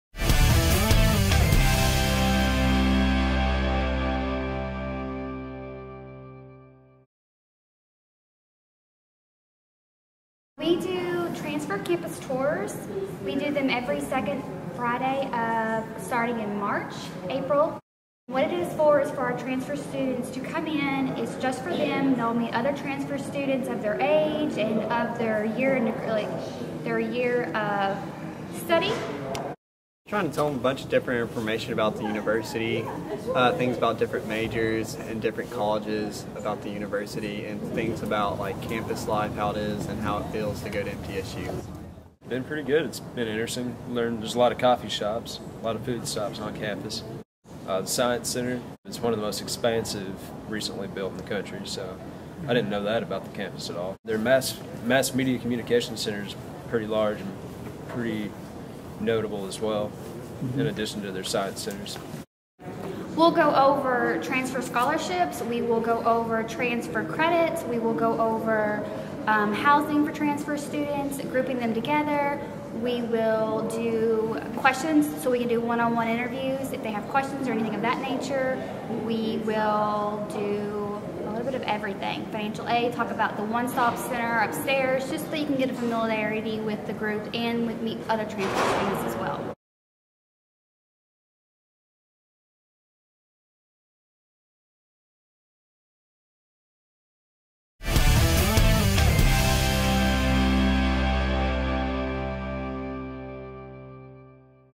MTSU launches special Friday transfer tours to recruit students
People visited MTSU from as far away as the Atlanta, Georgia, area and Knoxville, Tennessee. Tullahoma, Five Points community in Lawrence County, Chapel Hill, Columbia, Greenbrier and Murfreesboro were other Tennessee hometowns for the campus guests.

Prospective students, most accompanied by parents or siblings, became the initial group to visit MTSU Friday (Feb. 13) in the first of three special Friday tours to recruit students wanting to transfer from community colleges and four-year universities.

It marks the start of a bold initiative by MTSU to recruit transfer students.

“This is for transfer students to come in and meet other students their age and their year of study,” said Audrey Welch, transfer enrollment coordinator, who provided a 20-minute introduction utilizing PowerPoint and a video.

“We will do a campus tour with them,” Welch added. “We will do a credit evaluation, We’ll go over transfer scholarships, transfer credits, housing for transfer students, grouping them together. We will answer questions and do one-on-one interviews. We’ll talk about financial aid and about the (MT) One Stop center. We’ll do a little bit of everything.”

The MT One Stop, a one-stop shop to help meet an array of students’ enrollment management needs, is located on the second floor of the Student Services and Admissions Center, 1860 Blue Raider Drive. The transfer tours begin in Room 110 in the same building.

Future transfer-specific tours will be held starting at 9:30 a.m. Friday, March 13, and Friday, April 10.